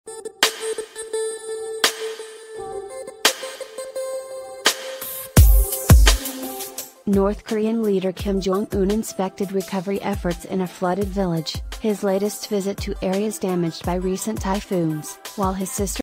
North Korea: Kim Jong Un tours flood-hit town with sister
North Korean leader Kim Jong Un inspected recovery efforts in a flooded village, his latest visit to areas damaged by recent typhoons, while his sister reappeared in public for the first time in about two months, state media said on Friday. Kim praised the speed of reconstruction work in Kimhwa County in the country's southeast during the visit with Kim Yo Jong. The younger Kim, considered the de facto second in command, is believed to be in her early 30s and is the leader's only close relative with a public role in politics. She became prominent with her harsh rhetoric against South Korea.